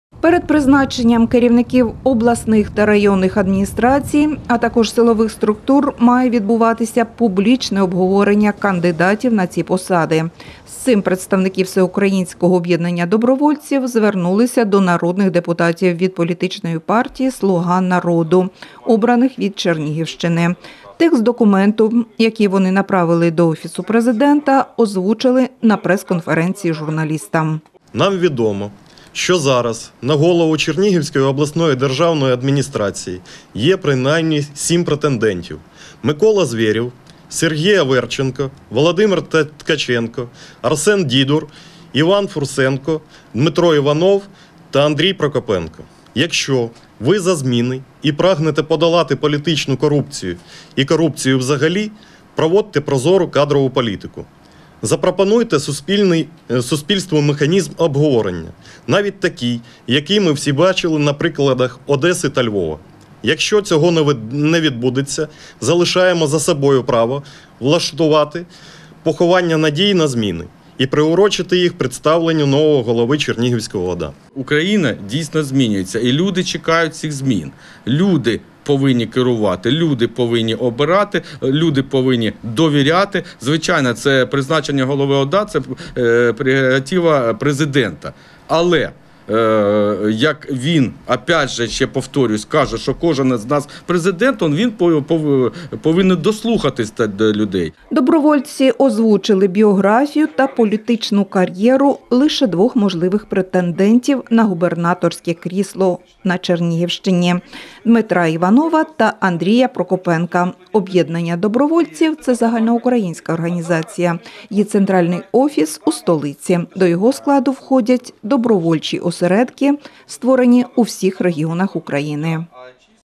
Хто претендує на крісло губернатора? | Телеканал Новий Чернігів
Всеукраїнське об’єднання добровольців озвучили свою позицію.